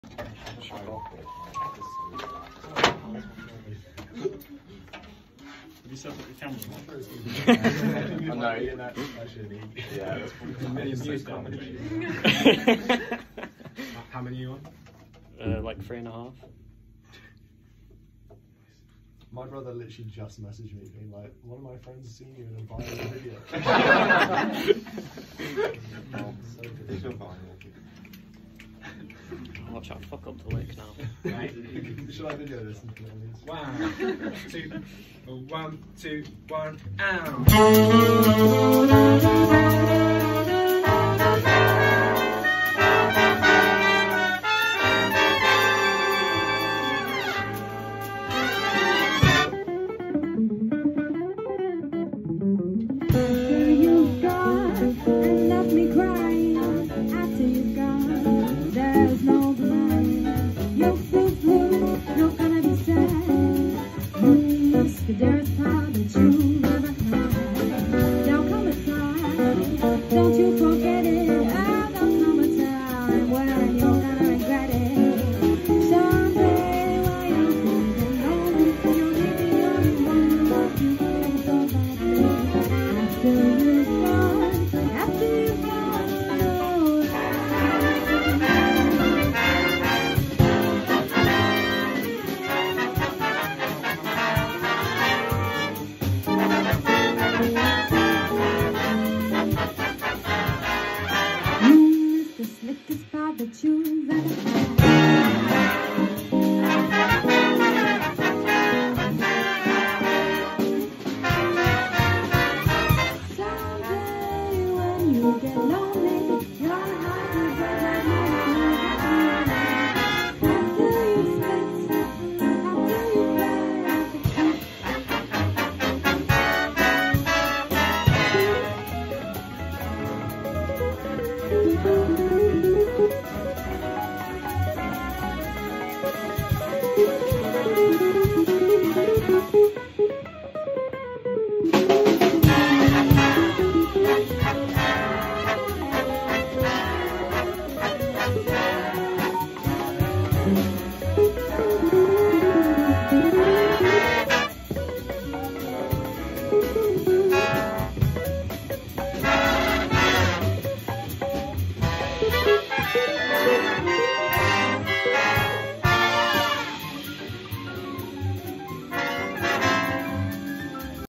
After You’ve Gone Viral - rehearsal footage
Going back to rehearsal after going viral on Instagram